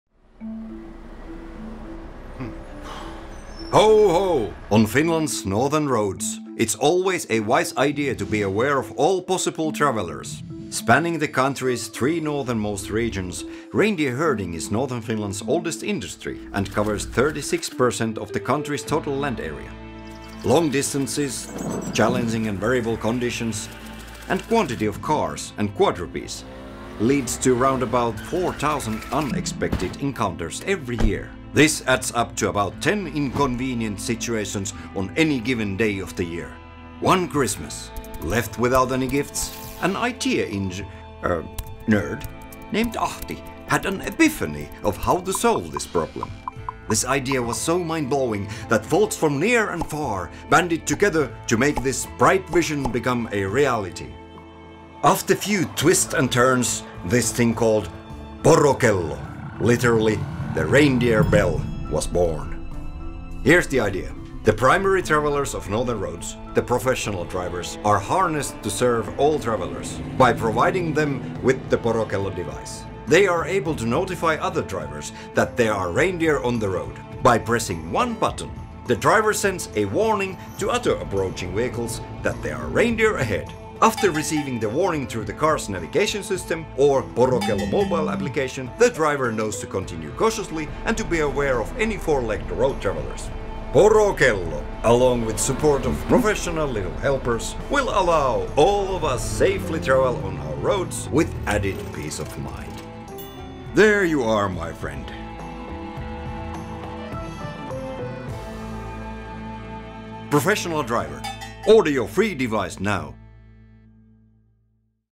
Porokello - The Reindeer bell: Reindeer app made in Finland
Centre for Economic Development, Transport and the Environment is developing in cooperation with e.g. Finnish Transport Agency, Reindeer herder's association in Finland, Paikkatieto Online Oy, V-Traffic, HERE and Milliemallie a service for road users "Porokello" to reduce reindeer accidents on the roads.